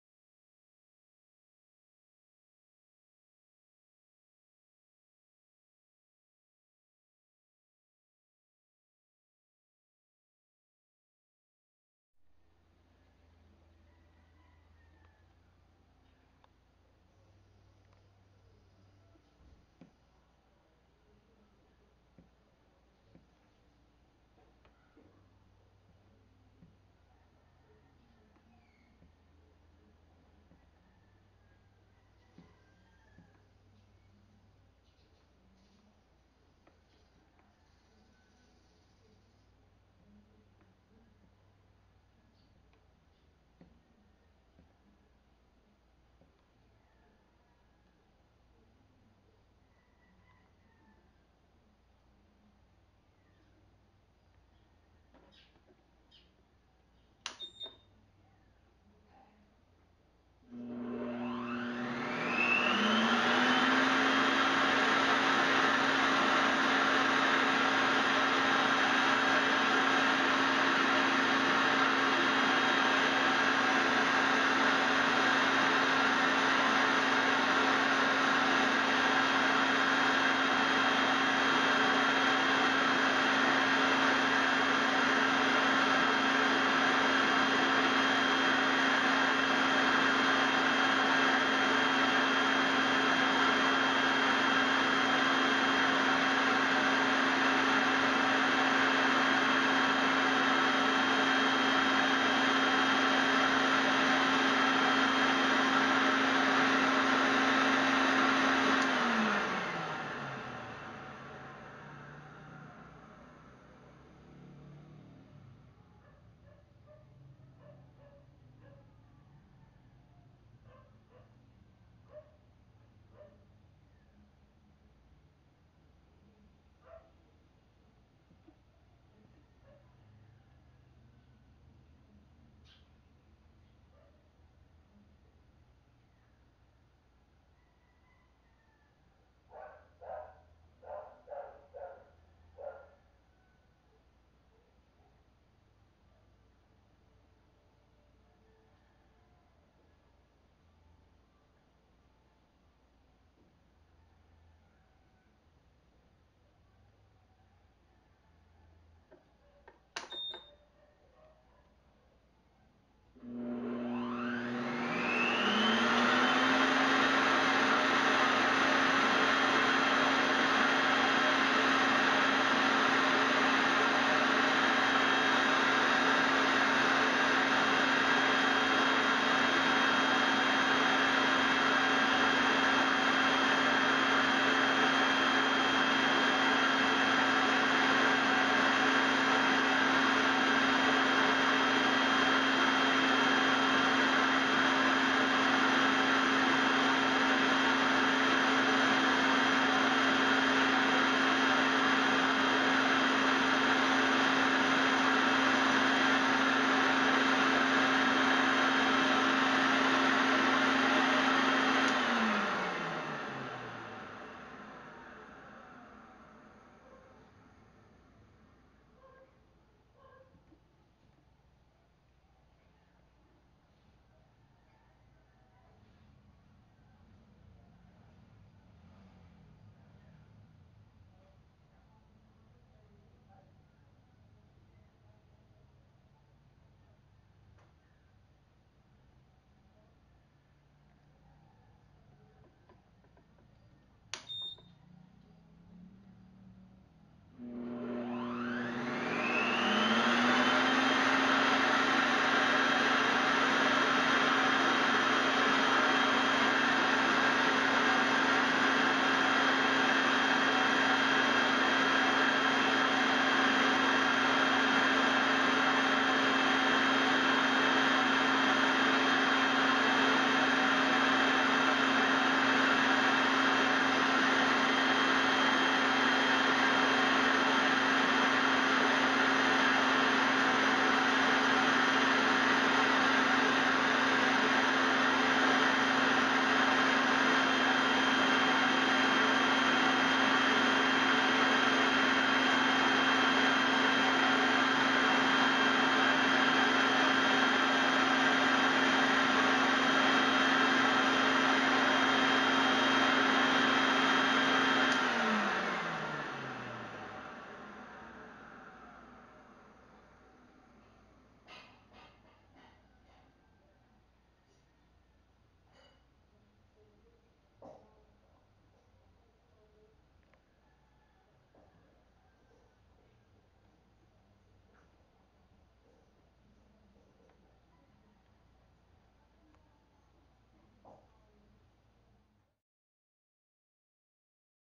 EPsolar Tracer 4215BN MPPT - On Float with 850W Load (Vacuum Cleaner)
EPsolar Tracer 4215BN MPPT SCC, 6x100W monocrystalline panels, 12-Volt System, 1000W Pure Sine Wave Inverter, 300Ah 12V Battery Bank (Motolite Solar Master Deep Cycle Batteries)

AC Load via PSWI = 850W Vacuum Cleaner